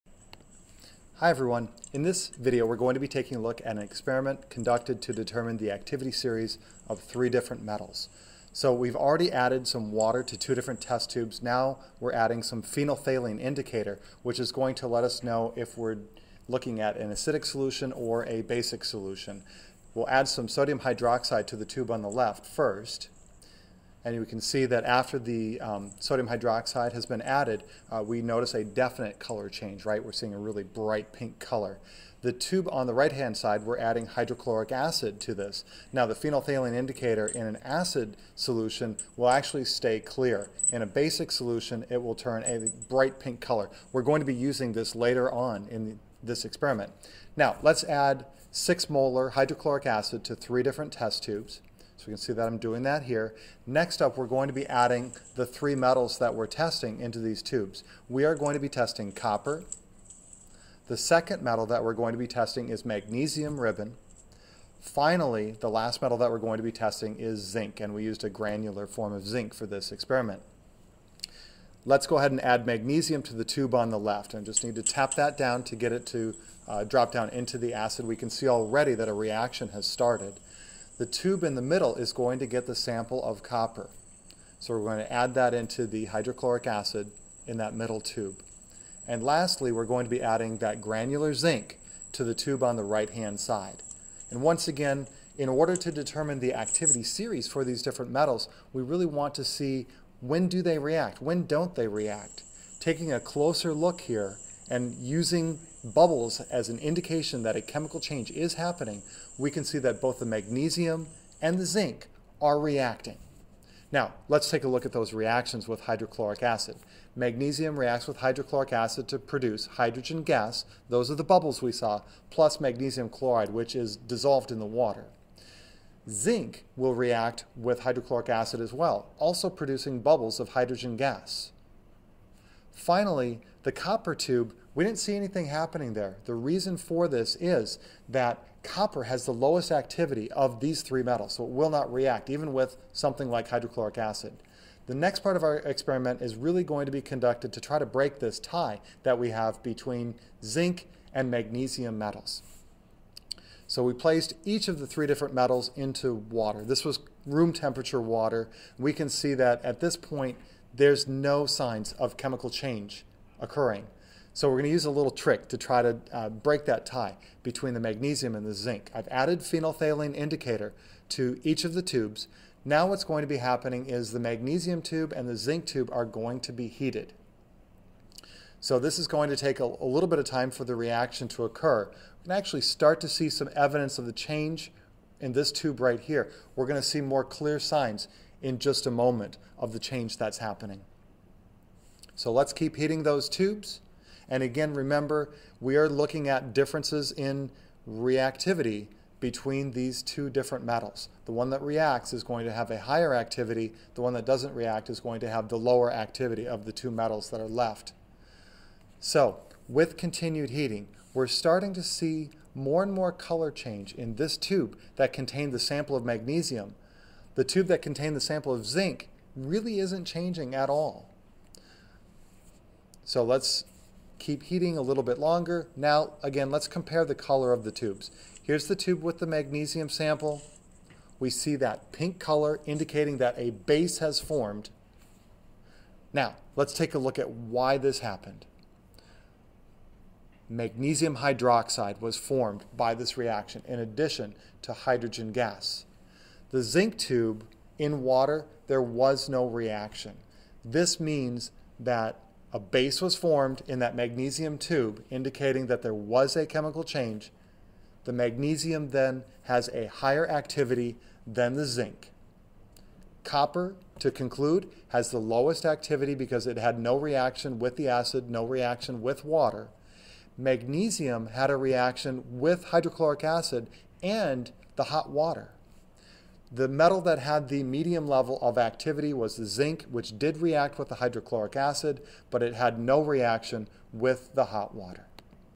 Determining the Activity Series of Metals - Chemistry Experiment - Mr Pauller
This video shows an experiment comparing the activity of three metals; Cu, Zn and Mg.  The metals are first tested hydrochloric acid,  and then hot water to determine their levels of activity.